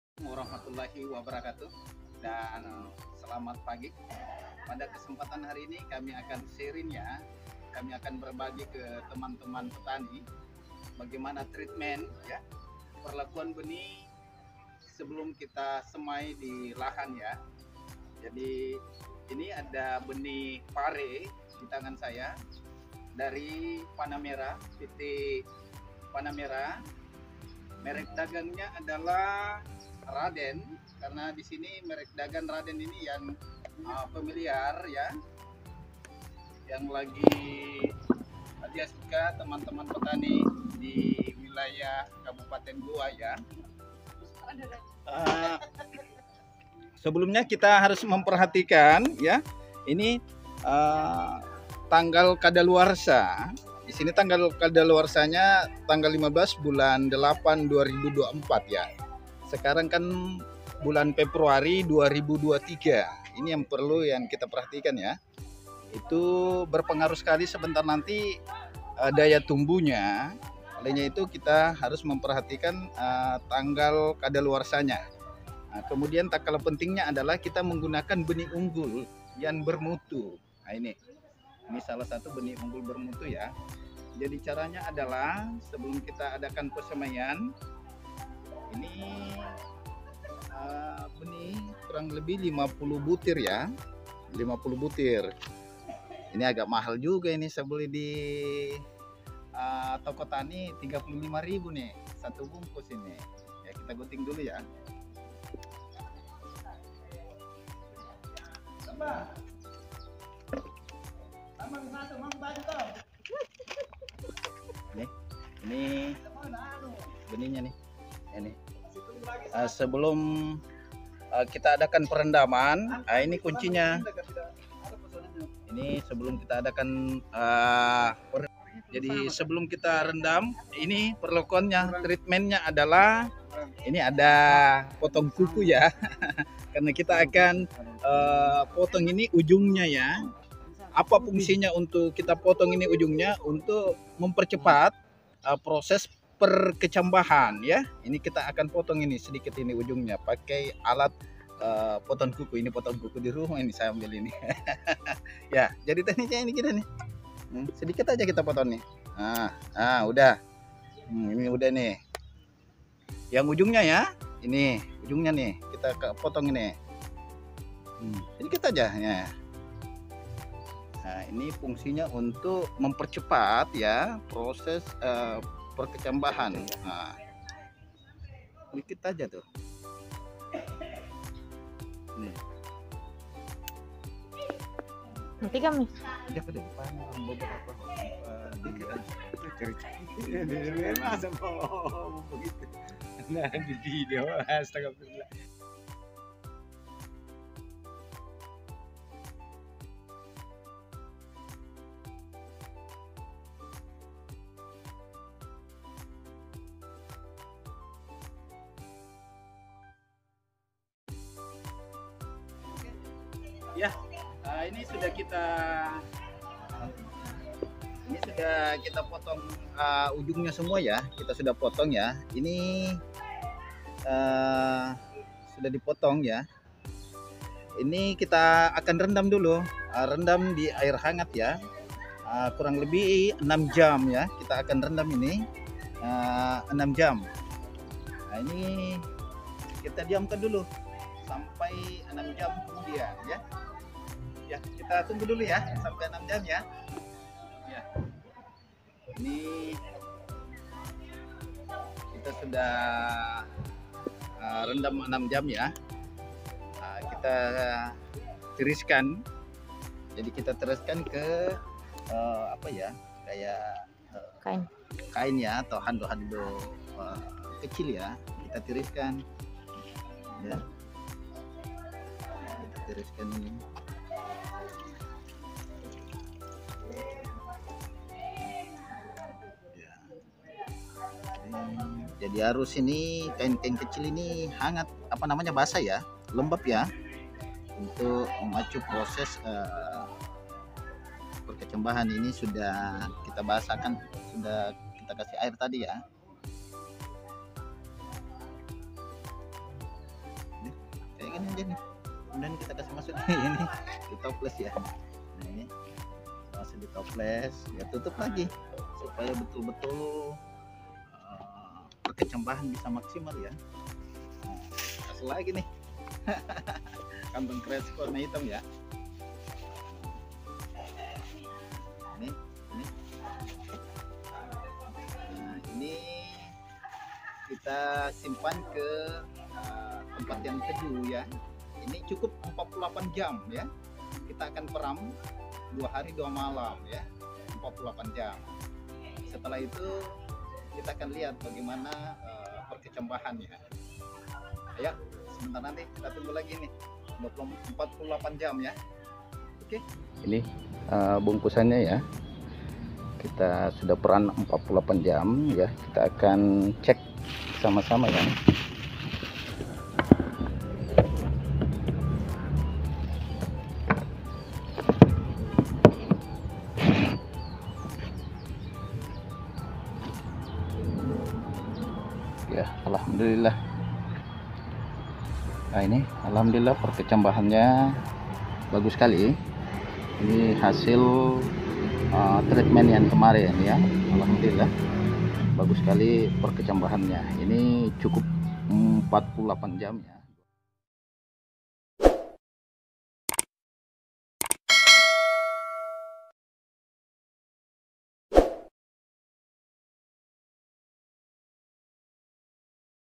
Rahasia‼️Mempercepat Perkecambahan Bibit Pare ‼️
Seed Treatment/Perlakuan Benih sebelum diadakan perendaman Bibit Pare/Paria adalah memotong ujung biji yang menonjol dengan menggunakan alat potong kuku
Tujuannya adalah untuk mempercepat proses Perkecambahan
Dan untuk lengkap mari kita simak di Tutorial ini mudah mudahan bermanfaat bagi Teman-teman Petani Pare/Paria